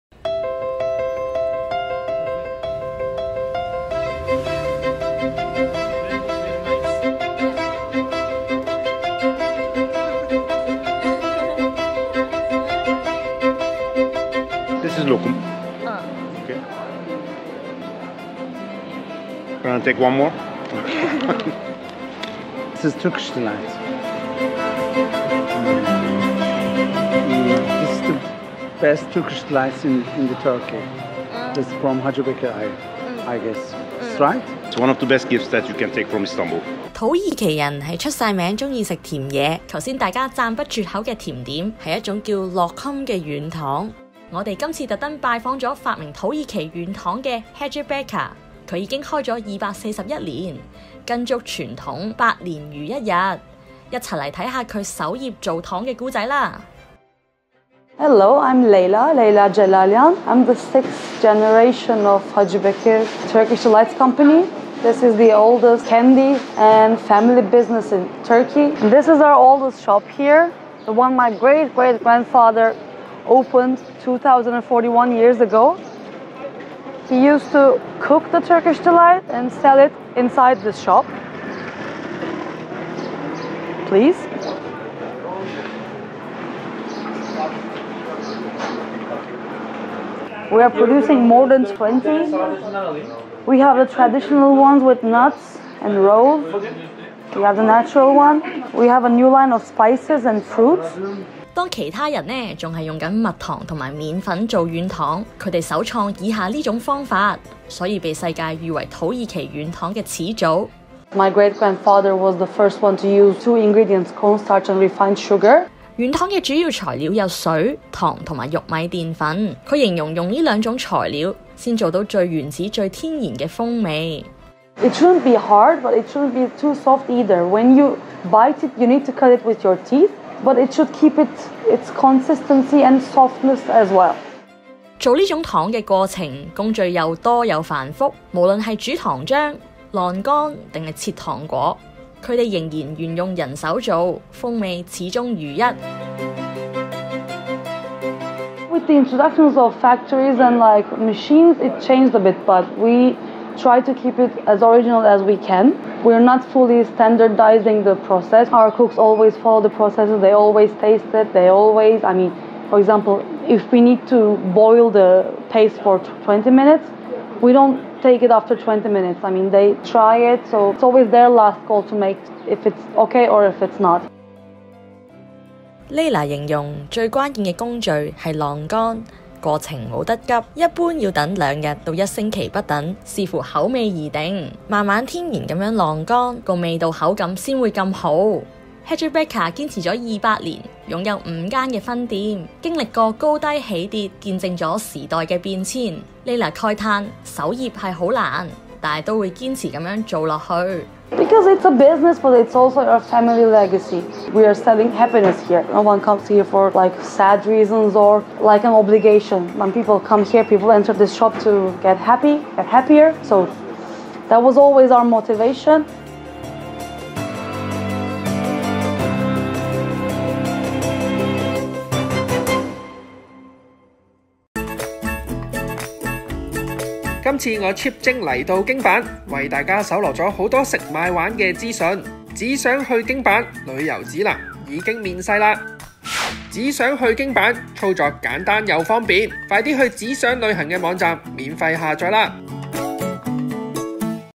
【伊斯坦堡6】 土耳其最老軟糖店 專訪第六代傳人：做糖為咗分享快樂！
土耳其諺語有云：「如果你吃得多甜食品，說的話都特別動聽。」土其耳人愛甜。由飛機上看到滿瀉的甜品餐車已感受到。或許因為土耳其人太迷信，認為吃糖能說出甜言蜜語，分享快樂，所以在伊斯坦堡有很多糖果店。專賣土耳其軟糖的店舖當中，最響亮的名字就是Hacı Bekir。